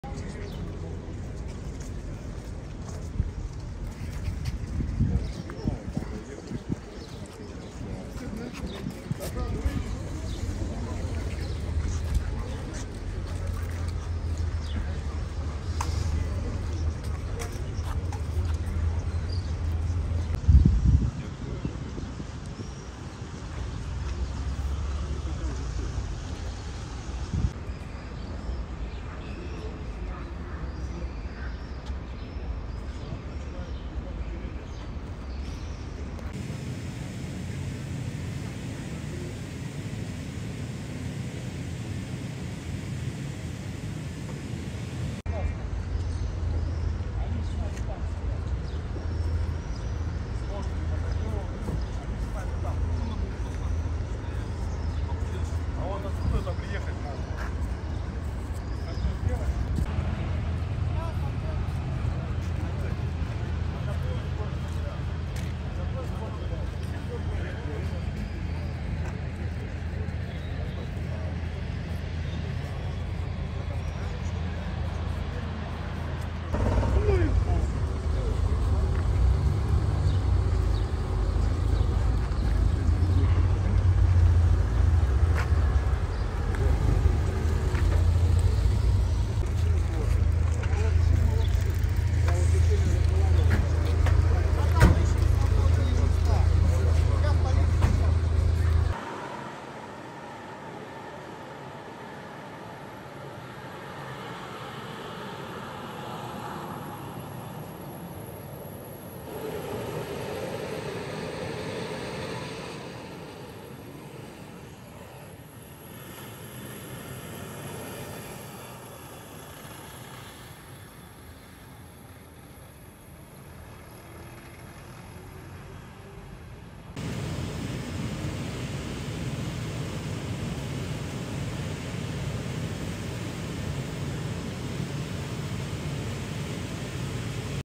SCENE: Residents in Russia's Rostov-on-Don wake up to Prigozhin's Wagner 'armed mutiny'
Residents of Rostov-on-Don looked on as mercenaries from Wagner's Private Military Group (PMC) took over a main street in the southern Russian city on Saturday (June 24) after the rebellious Russian mercenary chief Yevgeny Prigozhin said he had taken control.